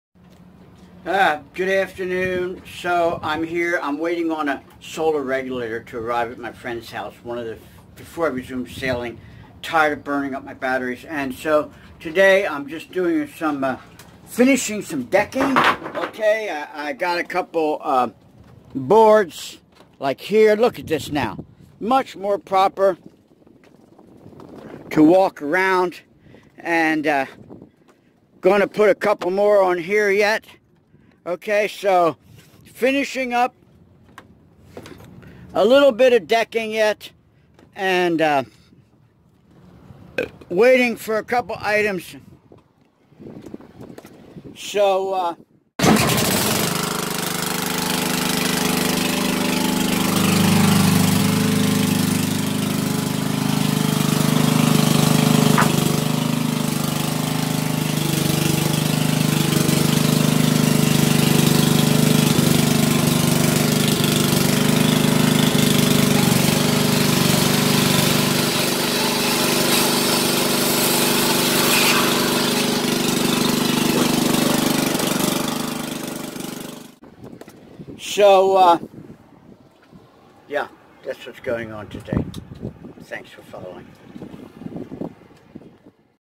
Video 39 5/4/20 'Make the Cut'
Jonathan's finishing some carpentry before sailing